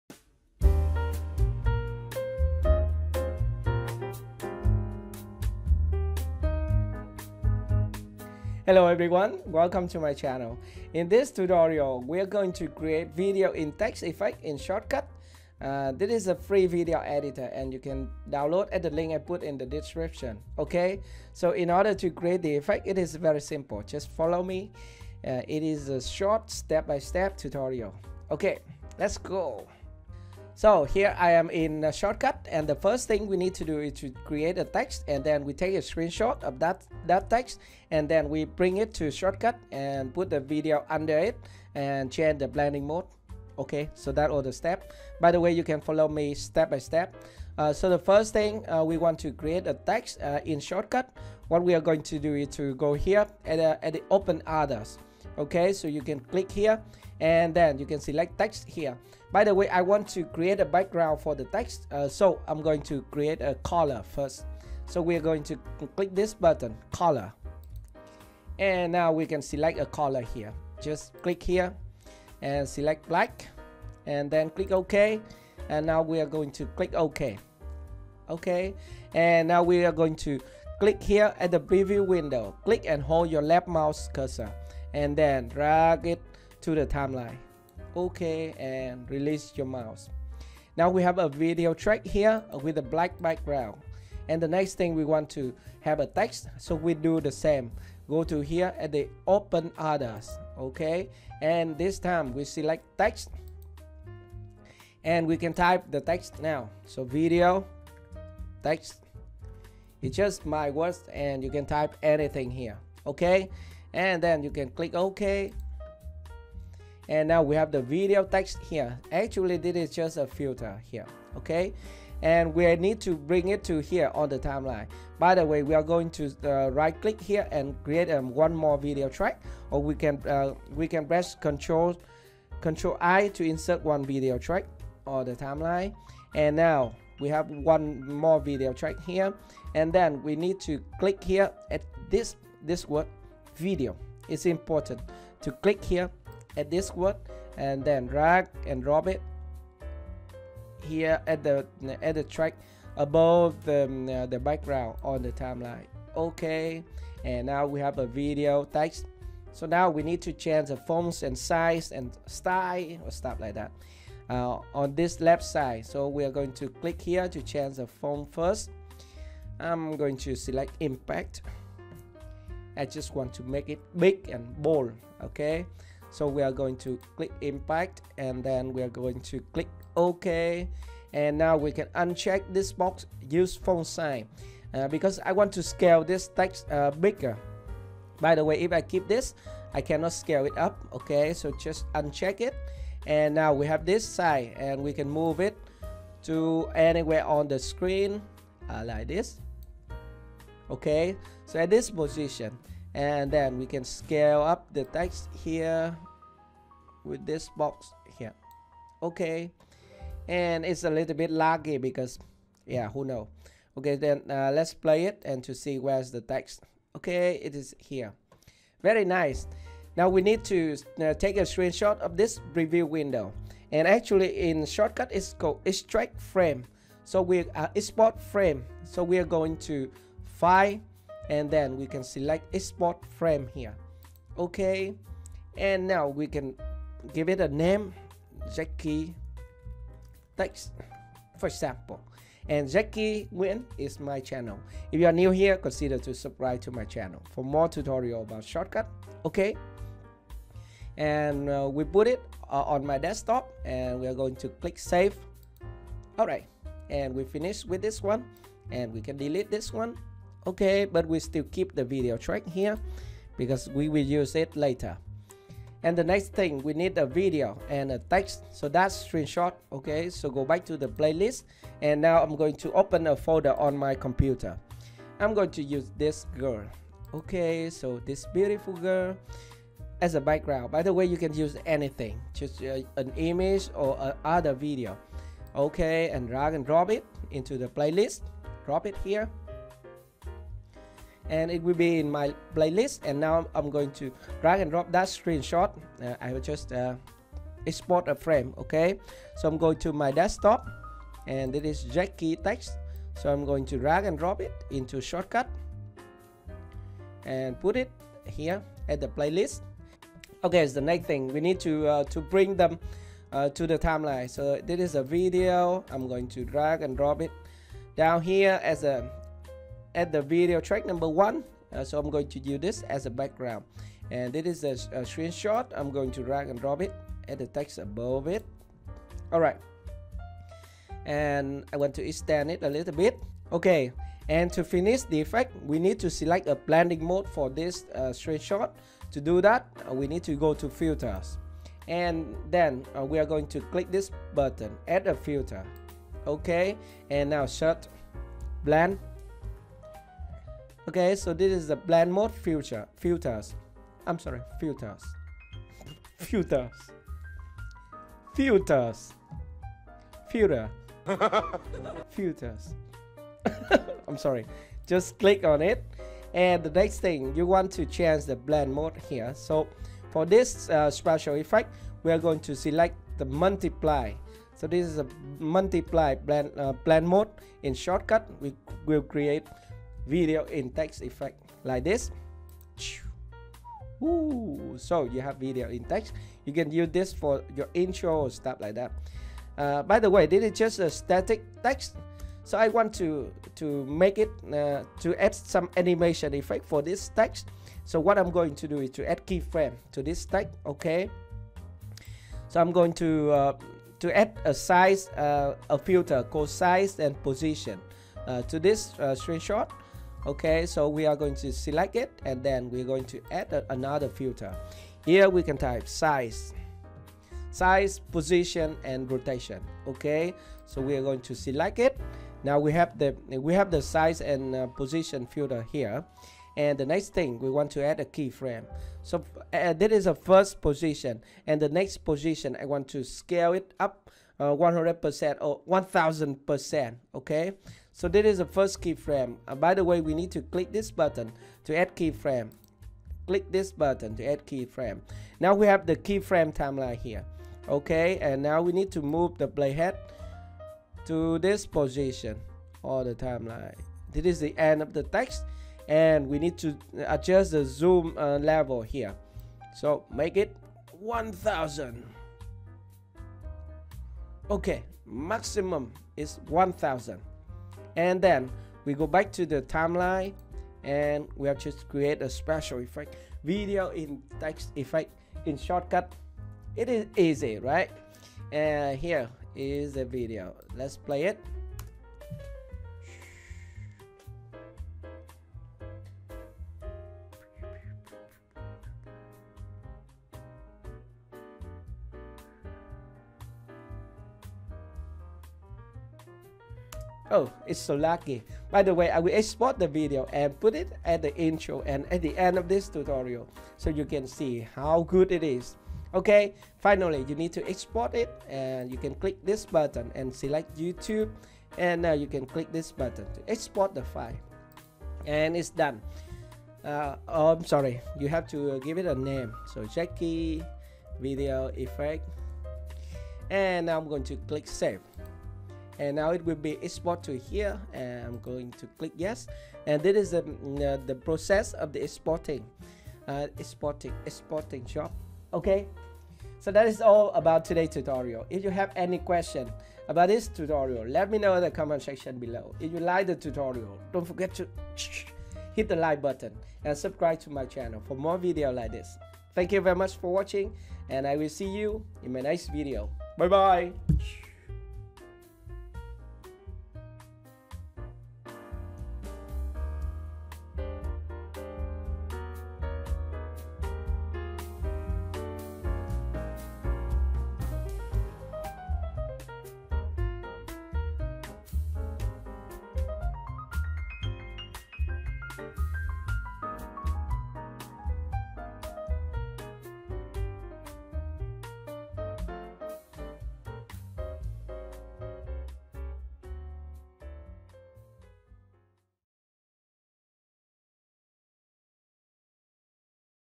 Shotcut Intro Effects - Video In Text Tutorial For Beginners
This tutorial shows you how to create a video intro effect in Shotcut using Video in text technique.